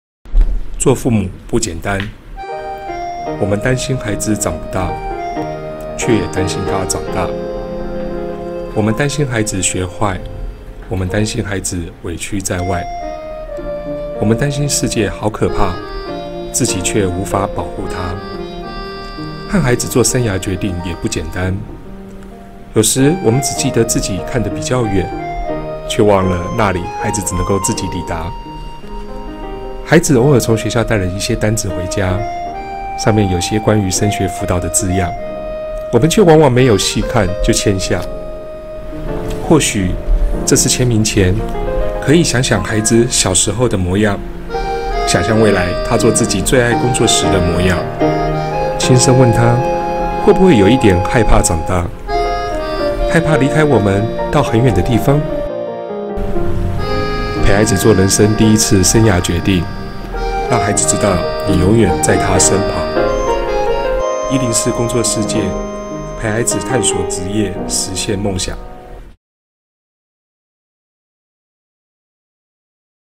陪孩子做人生第一次生涯決定
升學考試的季節都讓父母與孩子深感焦慮，
這一次，先別急著簽名，讓我們先聽聽孩子說他想說的話。

我們都擔心孩子的未來，但決策卻又往往下得太快。
我們不捨孩子將來吃苦，卻沒發現敢為夢想吃苦的他，其實比我們都還堅強。

104工作世界是一個陪伴孩子探索真實工作世界的網路公益平台。要陪孩子探索職業 x 實現夢想 。